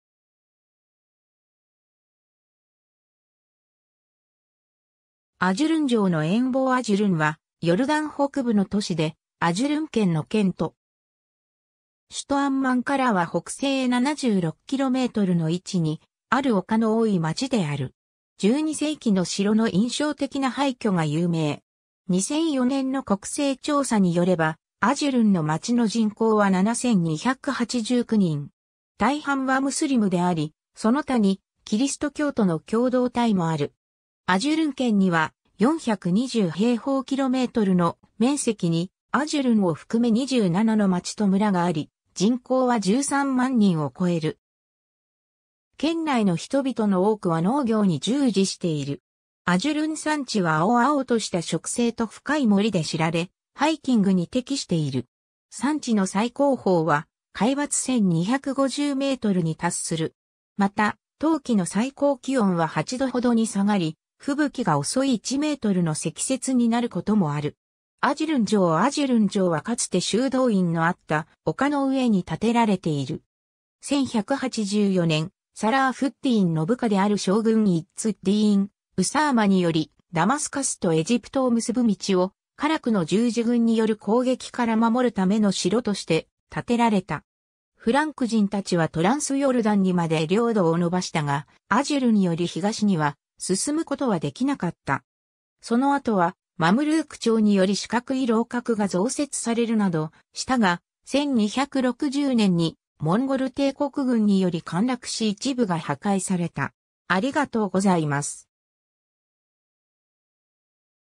アジュルン
アジュルン城の遠望 アジュルン（アラビア語: عجلون‎アジュルーン, Ajlun, Ajloun, Ājlūn）はヨルダン北部の都市でアジュルン県の県都。
首都アンマンからは北西へ76kmの位置にある丘の多い町である。
12世紀の城の印象的な廃墟（アジュルン城）が有名。
 2004年の国勢調査によれば、アジュルンの町の人口は7,289人。
大半はムスリムであり、その他にキリスト教徒の共同体もある。
アジュルン県には、420平方kmの面積にアジュルンを含め27の町と村があり、人口は13万人を超える。
県内の人々の多くは農業に従事している。
 アジュルン山地は青々とした植生と深い森で知られ、ハイキングに適している。
山地の最高峰は海抜1,250mに達する。
また冬季（1月）の最高気温は8度ほどに下がり、吹雪が襲い1mの積雪になることもある。
 アジュルン城 アジュルン城（カラアト・アジュルーン、アラビア語: قلعة عجلون‎, Qal'at Ajlun; またはカラアト・アッ＝ラバド、アラビア語: قلعة الربض‎, Qal'at ar-Rabad）はかつて修道院のあった丘の上に建てられている。
1184年、サラーフッディーンの部下である将軍イッズッディーン・ウサーマにより、ダマスカスとエジプトを結ぶ道をカラクの十字軍による攻撃から守るための城として建てられた。
フランク人（西洋人）たちはトランスヨルダン（ヨルダン川東岸）にまで領土を延ばしたが、アジュルンより東には進むことは出来なかった。
その後はマムルーク朝により四角い楼閣が増設されるなどしたが、1260年にモンゴル帝国軍により陥落し一部が破壊された。